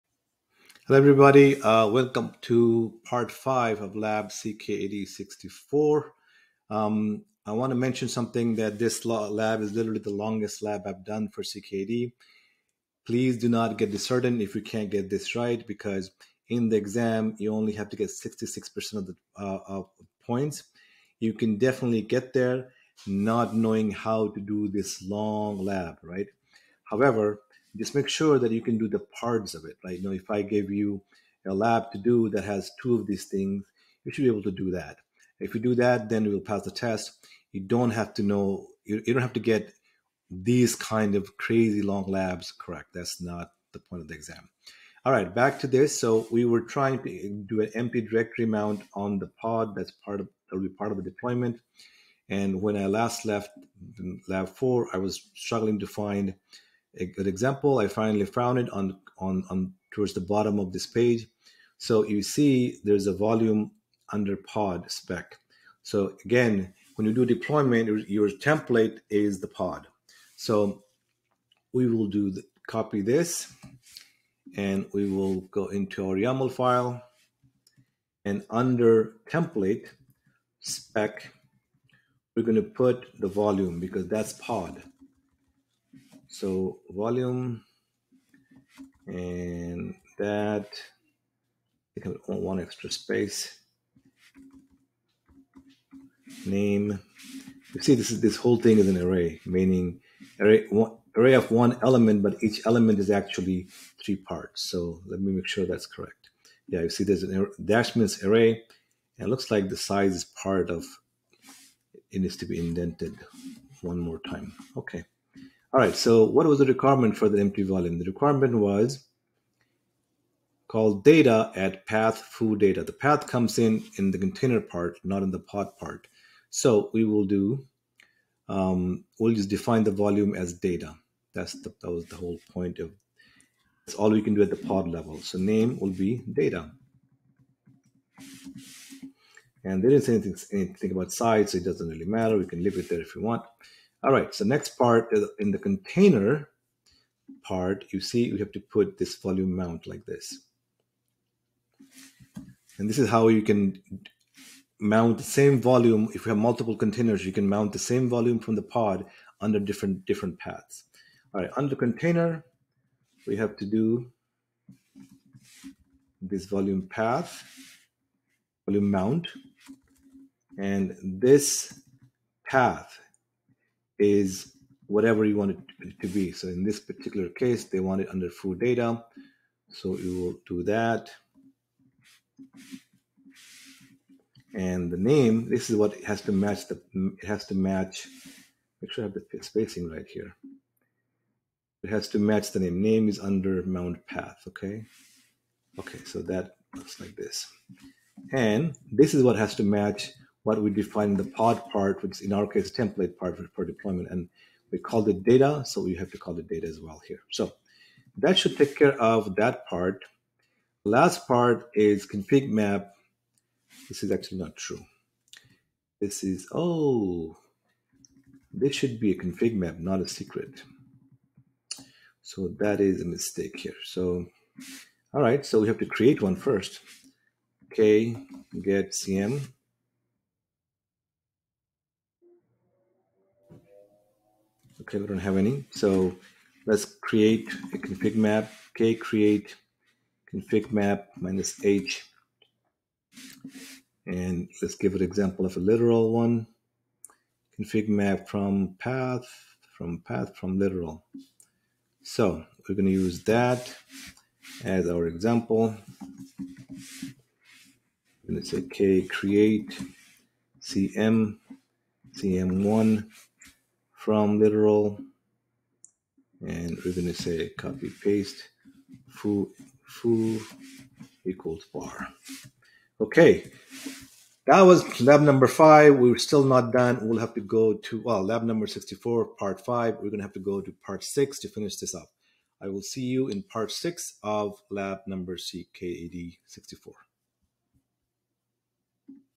CKAD Lab 64 Part 5 of 8 (Kubernetes Certification) deployment mount configmap & emptyDir and MORE
Requirements:

  Create a cm called cm1  values FOO=bar

  Create a deployment called foo-redis with the following parameters:
    - Name of the deployment should be redis using the redis:alpine image. It should have exactly 3 replicas
    - The container should request for .3 CPU.
    - The container should use the label app=foo
    - The container should expose the port 6379
    - Make sure that the pod is scheduled on the FIRST NODE
    - It should mount exactly 2 volumes:
      - An Empty directory volume called data at path /foo-data
      - A configmap volume called cm1 at path /foo-cm

SOLUTION:
apiVersion: apps/v1
kind: Deployment
metadata:
  name: foo-redis
  labels:
    app: foo
spec:
  replicas: 3
  selector:
    matchLabels:
      app: foo
  template:
    metadata:
      labels:
        app: foo
    spec:
      affinity:
        nodeAffinity:
          requiredDuringSchedulingIgnoredDuringExecution:
            nodeSelectorTerms:
              - matchExpressions:
                  - key: foo
                    operator: In
                    values: ["bar"]
      volumes:
        - name: data
          emptyDir:
            sizeLimit: 500Mi
        - name: cm1
          configMap:
            name: cm1
            items:
              - key: "cm1"
                path: "foo-cm"
      containers:
      - name: foo-conatiner
        image: redis:alpine
        resources:
          requests:
            cpu: "0.3"
        ports:
        - containerPort: 6379
        volumeMounts:
          - mountPath: /foo-data
            name: data
          - mountPath: /foo-cm
            name: cm1